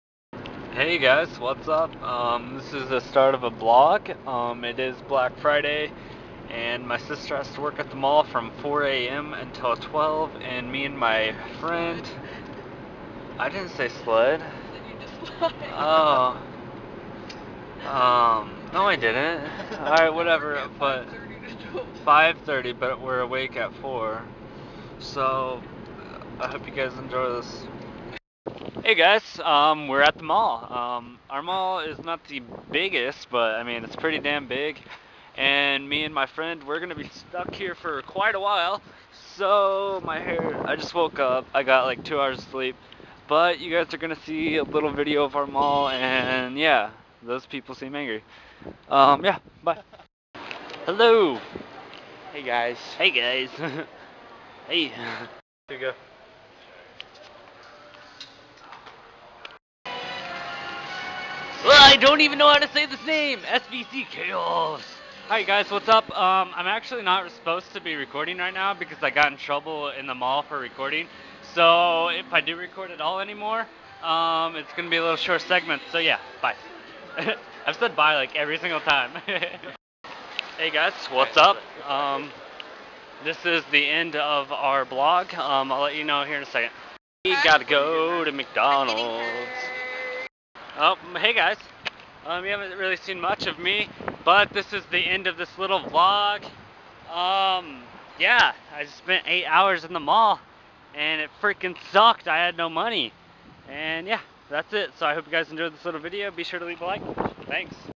BlackFriday VLOG! Stuck At Mall For 8 Hours
Hey guys here's a short little blog inside my mall on black friday, I was stuck their from 4am till 11am. I couldn't record much at all because in the very begging we got in trouble by cops saying we couldn't record anything. so yeah sorry for it being so short.

but enjoy!